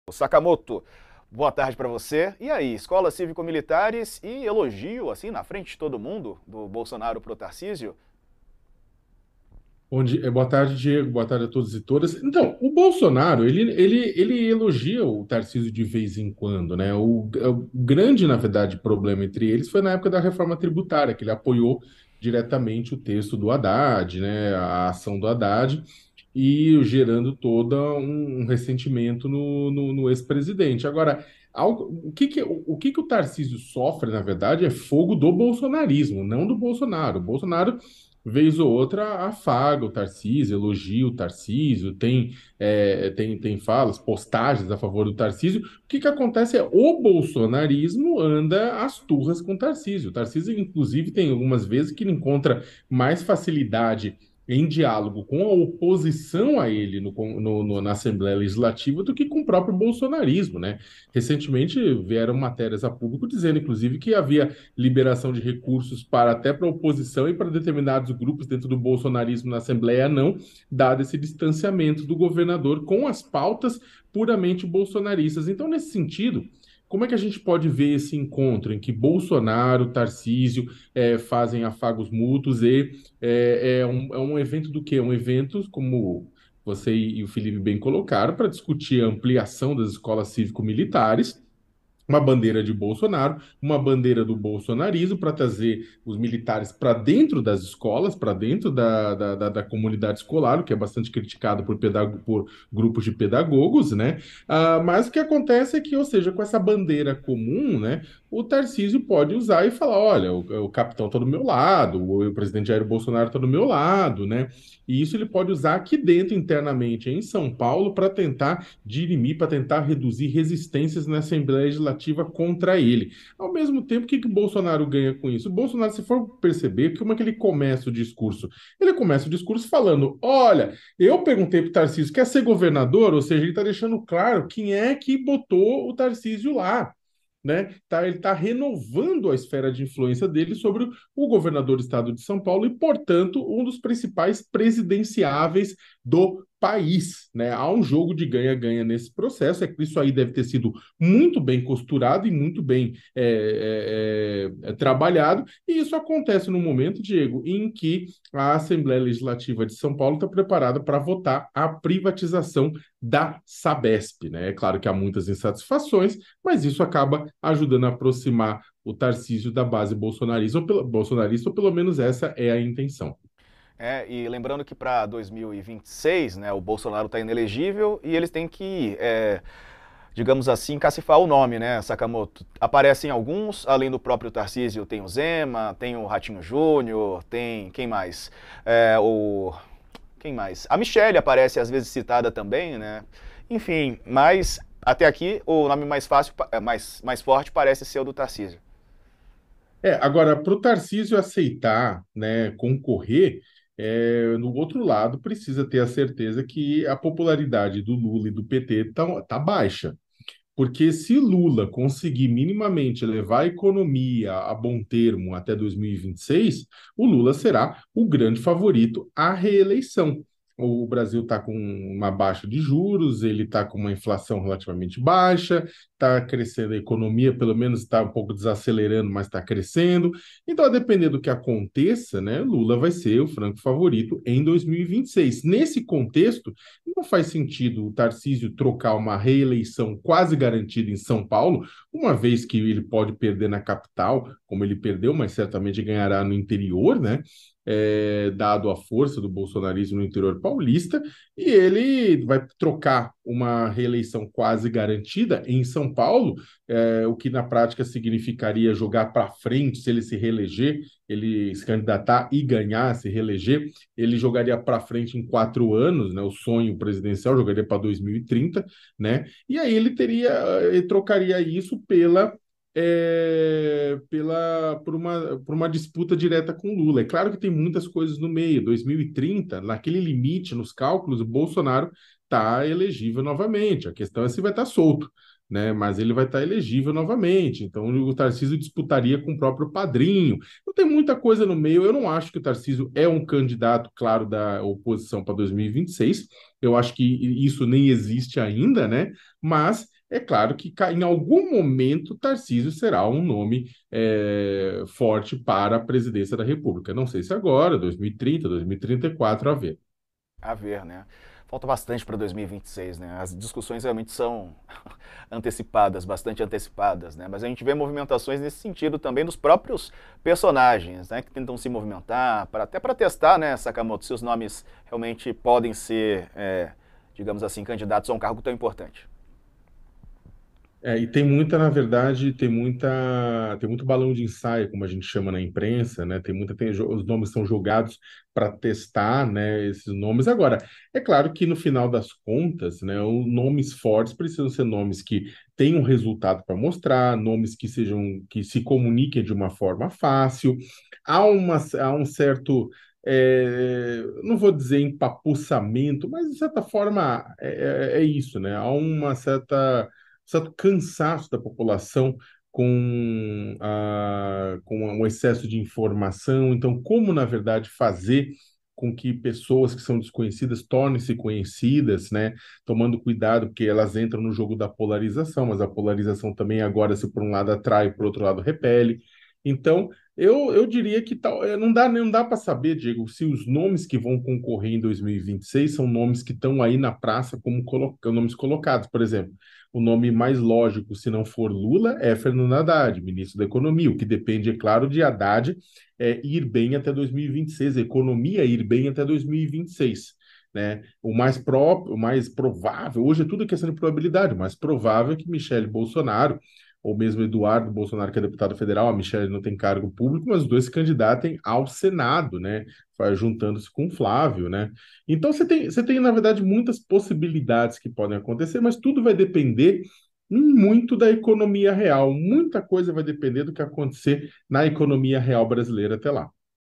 Bolsonaro e Tarcísio se afagam por que há jogo de ganha-ganha | Leonardo Sakamoto
Leonardo Sakamoto comenta o encontro entre o ex-presidente Jair Bolsonaro e o governador de São Paulo, Tarcísio de Freitas.